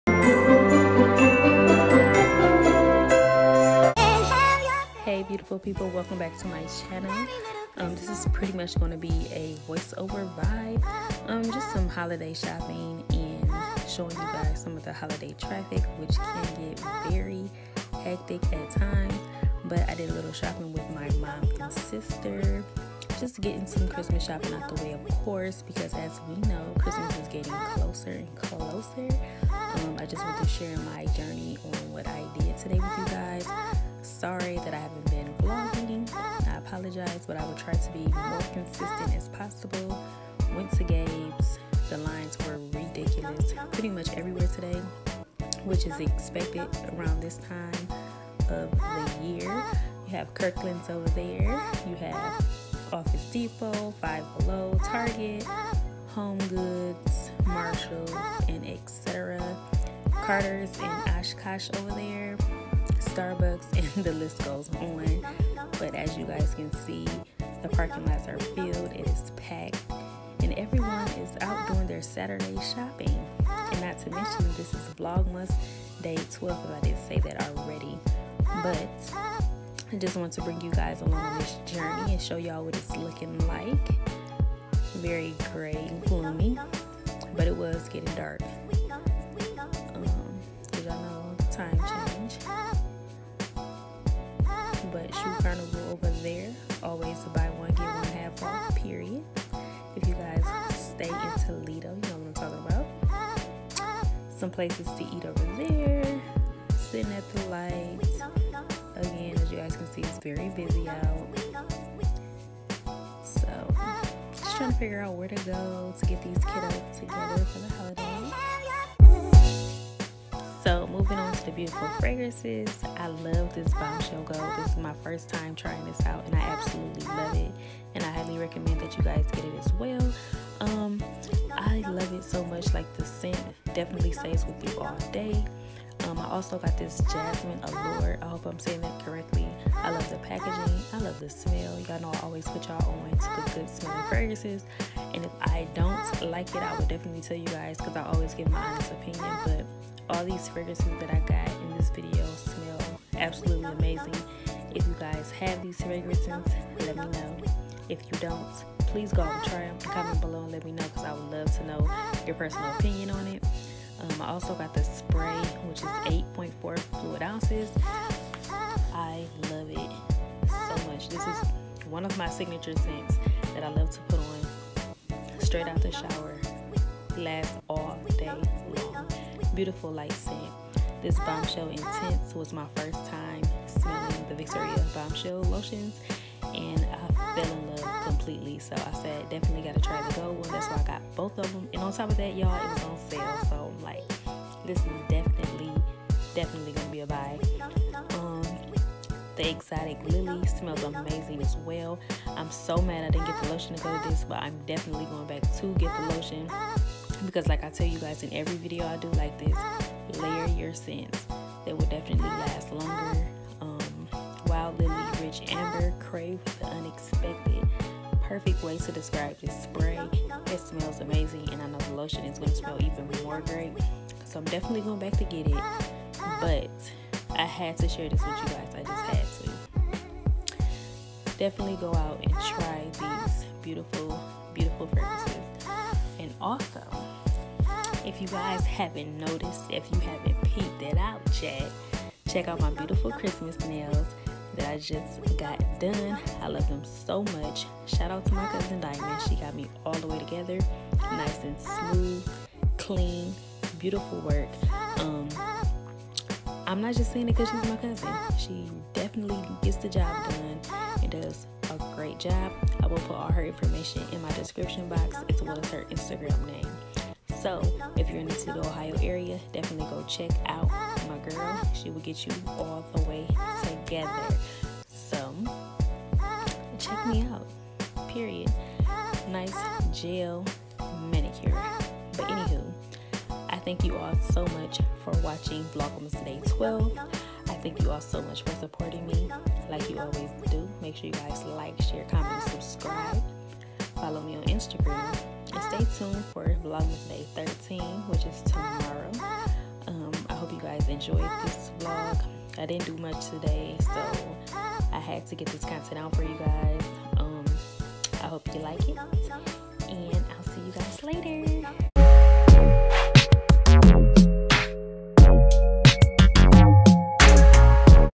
Victoria’s Secret Bombshell Lotions, Shopping + More | Vlogmas Day 12
Sorry for the late upload ...Vlogmas day 13 will be posted later on today!!!